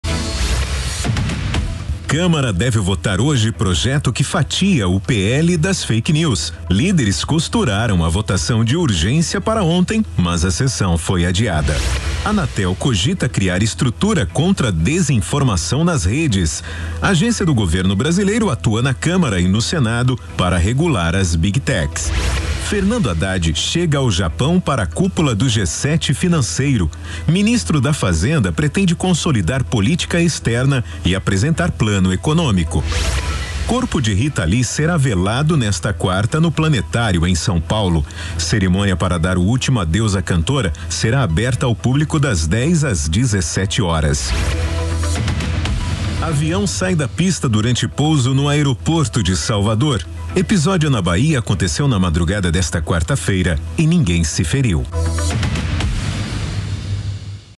Fernando Haddad chega ao Japão para cúpula do G7 Financeiro | BREAKING NEWS
PL das Fake News deve ser votada, nesta quarta-feira (10), na Câmara; Anatel cogita criar estrutura contra desinformação nas redes sociais; ministro da Fazenda, Fernando Haddad, chegou ao Japão para participação na cúpula do G7 Financeiro; Rita Lee será velada, hoje (10), no Planetário no Parque do Ibirapuera, em São Paulo; avião sai da pista durante pouso no aeroporto de Salvador. Episódio aconteceu nesta madrugada e não deixou feridos.